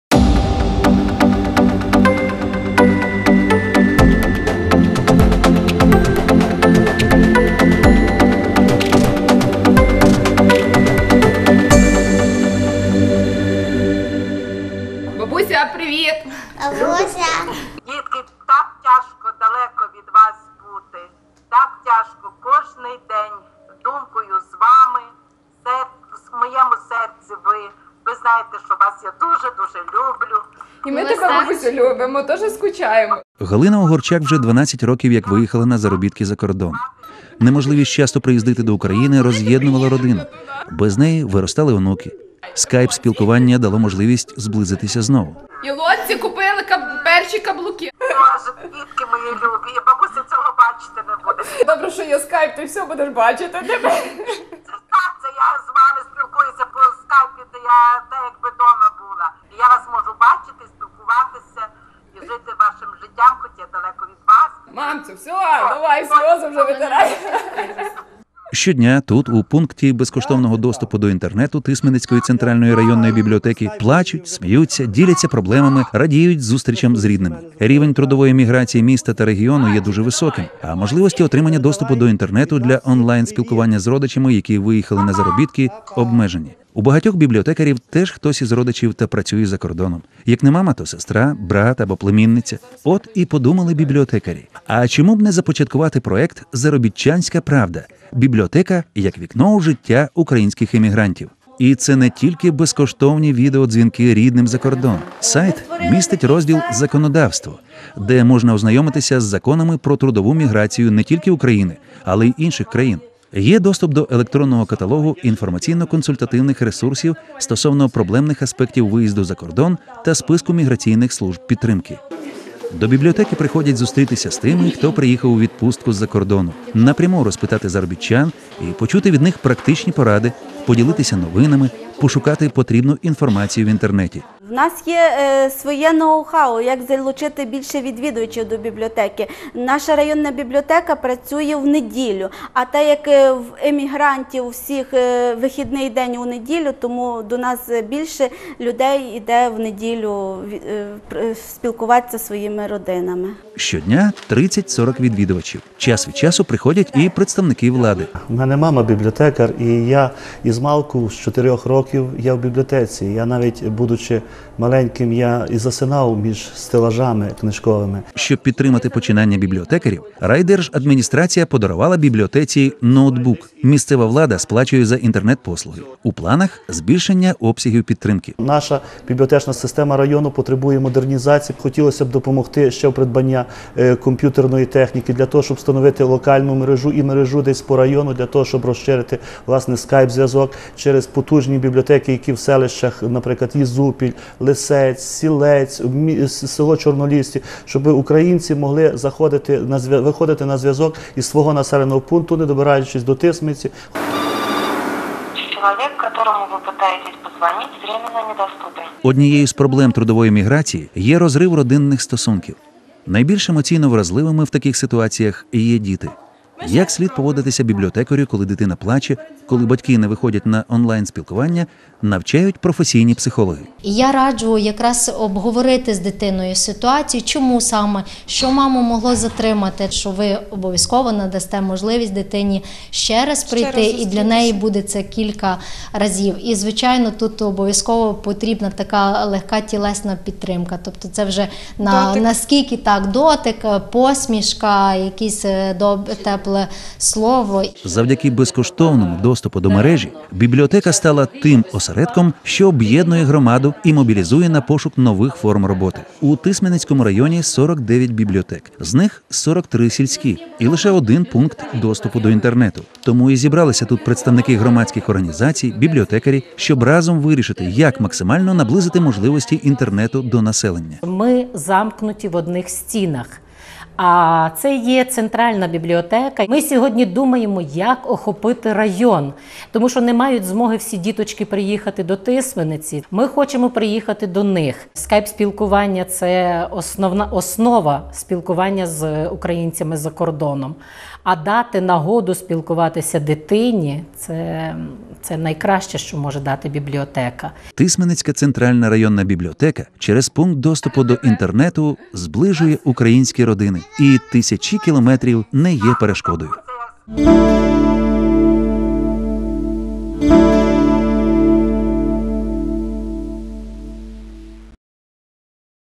Library in Ukraine Connects Families Online
Library in Ukraine helps labor migrants connect with families online using Skype.

Through a partnership between the Bill & Melinda Gates Foundation, IREX, the Ukrainian Library Association (ULA), local and national governments, and libraries throughout the country, Global Libraries-Bibliomist fosters the development of a modern public library system in Ukraine as a step towards improving the socioeconomic conditions of its citizens.